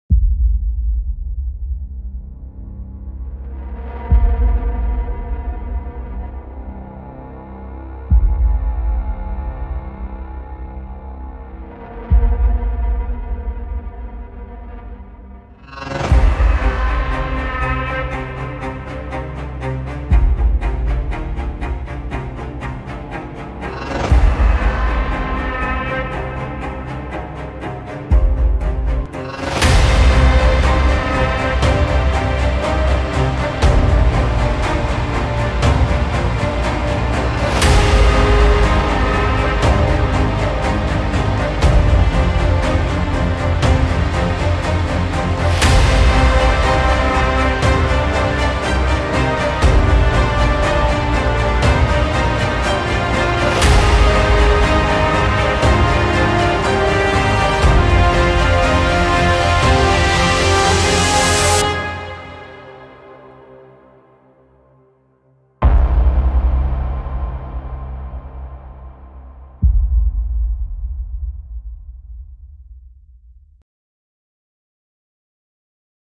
Concours montage vidéo - Pub BMW Serie 1
Petit concours vidéo de "Creator HUB" auquel je participe avec ce montage !